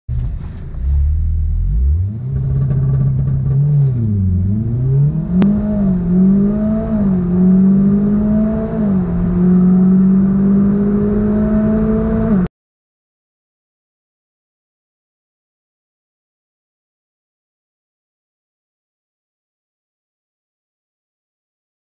1/8th mile using launch control. 1.8t DQ250 DSG Ignitron ECU.
Only running 1.2 bar boost as still on stock DSG gearbox mapping .Not a very aggressive launch with very little wheel spin.